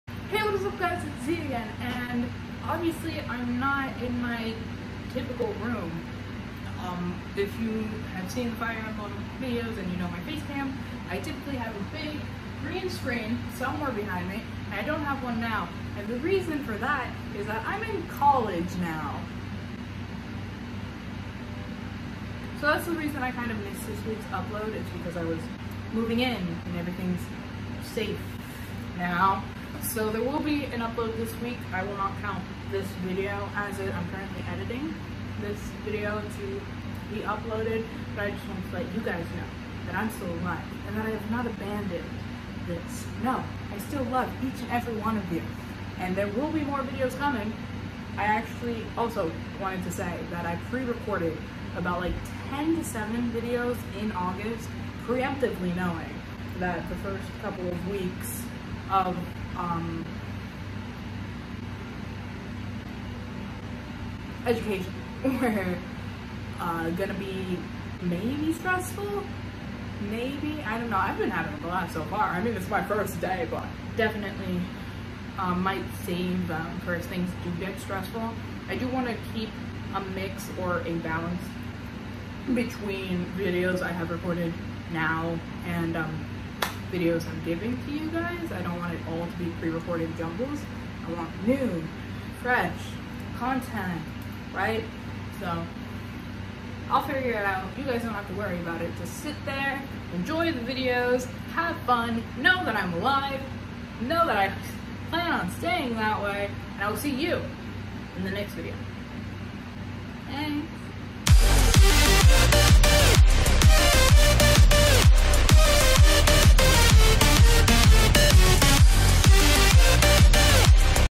No I'm not dead[Channel Update!]
My loving gap year has come to it's beautiful end so now we journey into the forbidden college realms.
Fear not for this gamer brought her entire fucking set up with her so LETS GO TEAM!!!